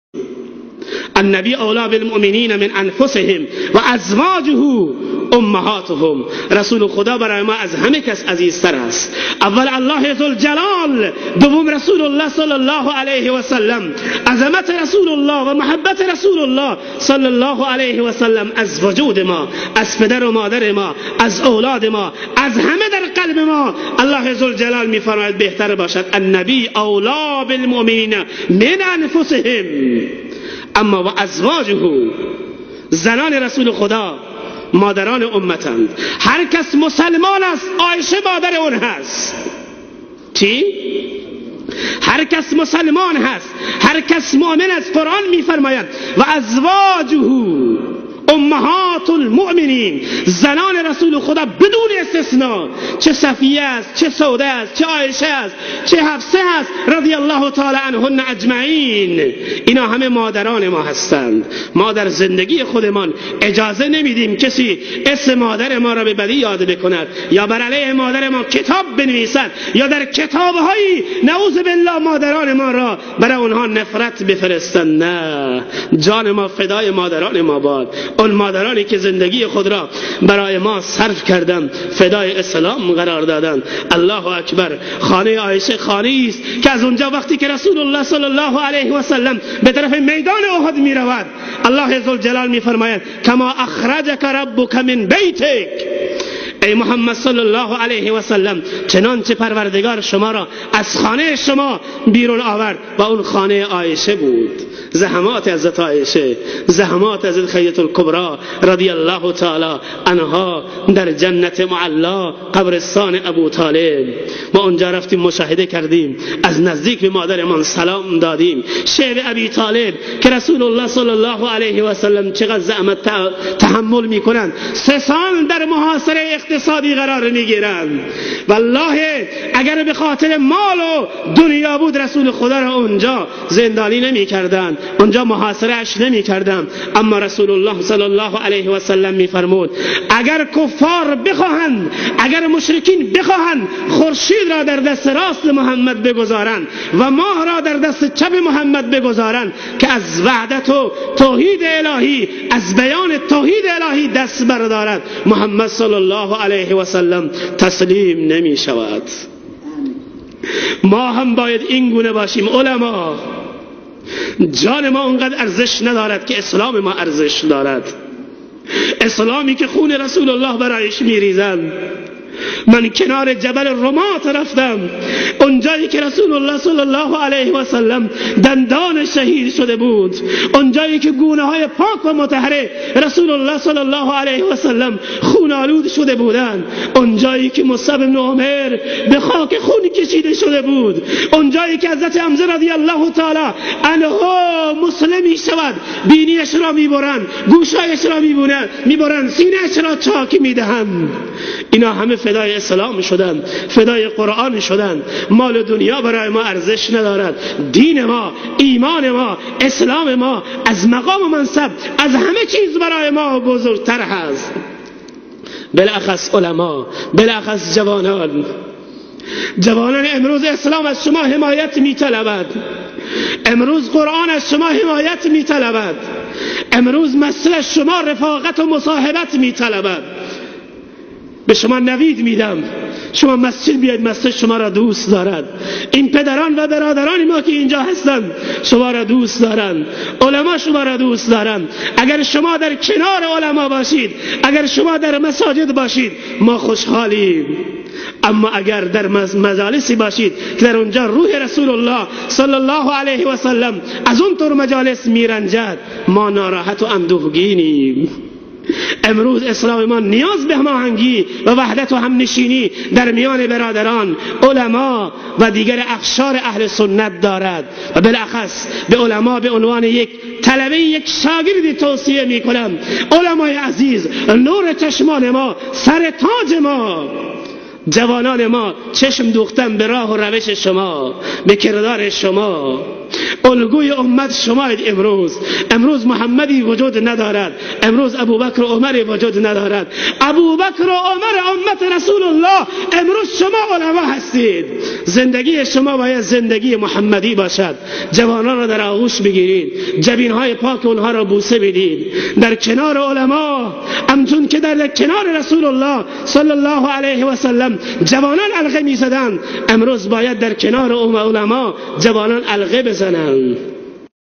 جایگاه امهات المومنین درنزد مومنین
ازواج رسول الله صلی الله علیه وسلم کسانی اند که خداوندمتعال قلوب انها راتزکیه نمود ودر حق انها فرموده.إِنَّمَا يُرِيدُ اللَّهُ لِيُذْهِبَ عَنْكُمُ الرِّجْسَ أَهْلَ الْبَيْتِ وَيُطَهِّرَكُمْ تَطْهِيرًا (33)احزاب.وبرای طهارت قلوب سایر اهل بیت رسول اکرم دعا نمود